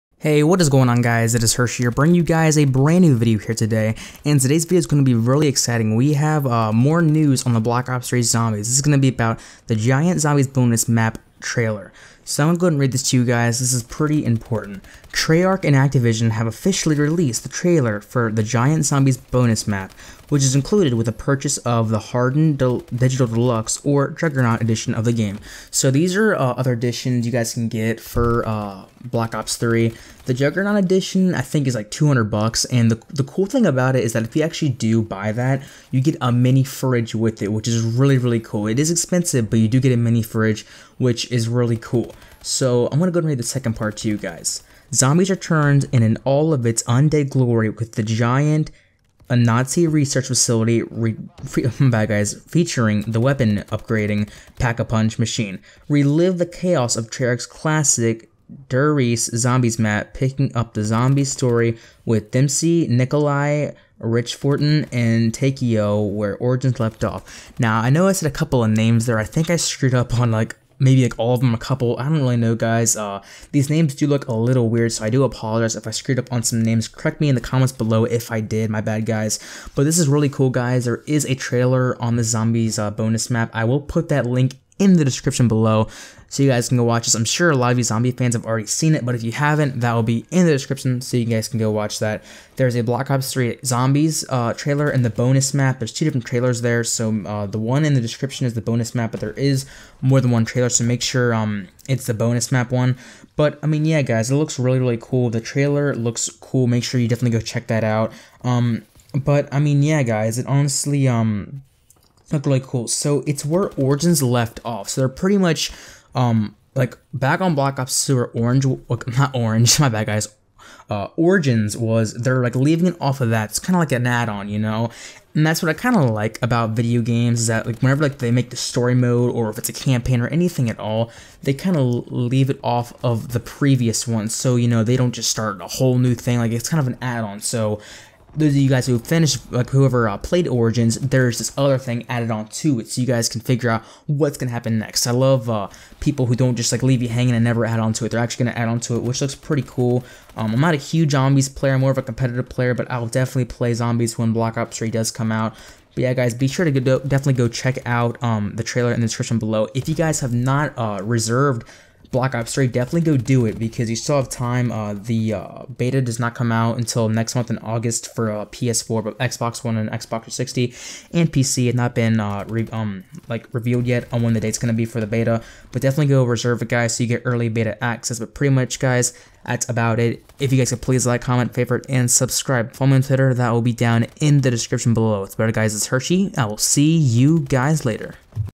Black ops 3 ''The Giant'' Zombies Bonus Map !
In this video i talked about the Black ops 3 giant zombies map. They posted a trailer on it and the map looks pretty cool.